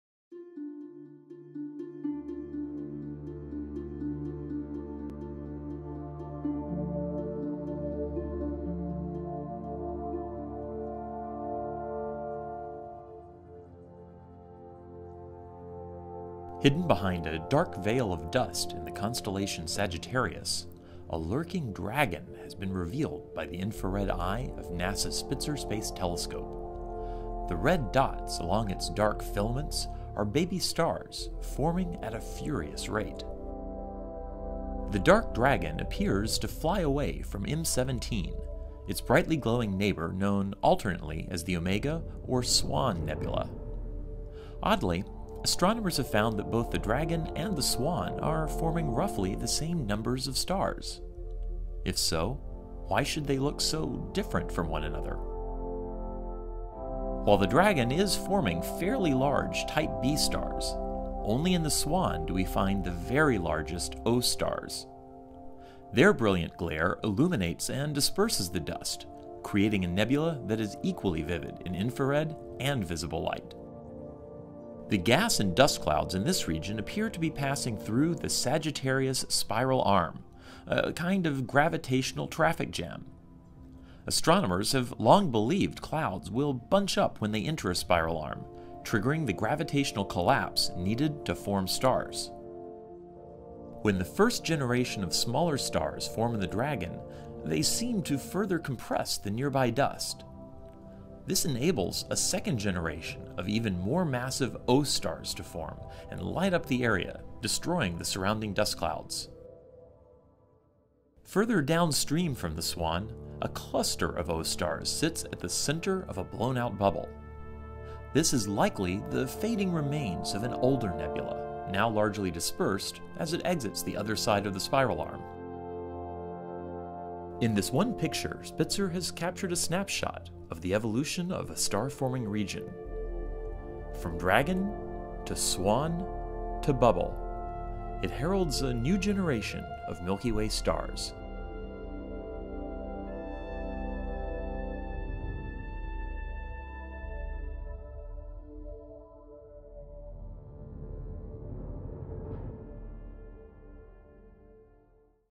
The Dragon and the Swan (Hidden Universe Gallery Explorer)
Hidden behind a dark veil of dust in the constellation Sagittarius, a lurking dragon has been revealed by the infrared eye of NASA's Spitzer Space Telescope. It gives us a glimpse into how spiral arms affect the formation of stars.